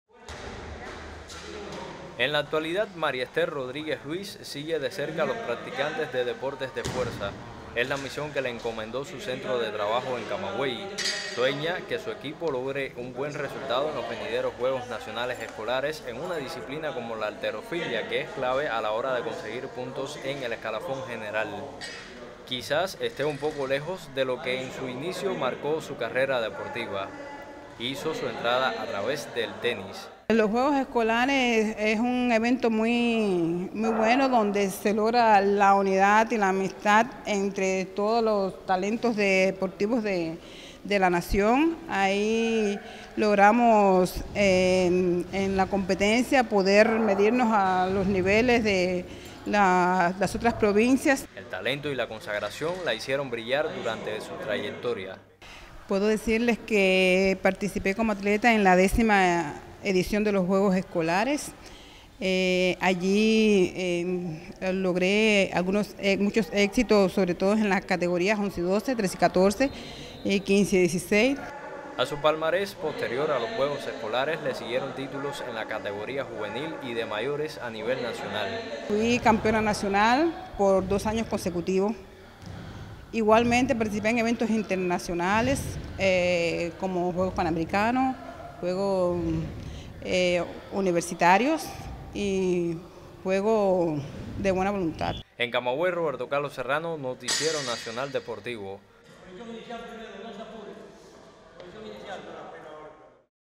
La historia de una de las glorias deportivas del tenis en Camagüey
A propósito de la cercana celebración de los Juegos Nacionales Escolares en su edición 60, en la provincia de Camagüey quedan participantes que en las primeras ediciones obtuvieron resultados que le permitieron trascender en todas las categorías.

Ese es el caso de la tenista María Esther Rodríguez Luis, quién contó su historia.

Cámara: Ángel García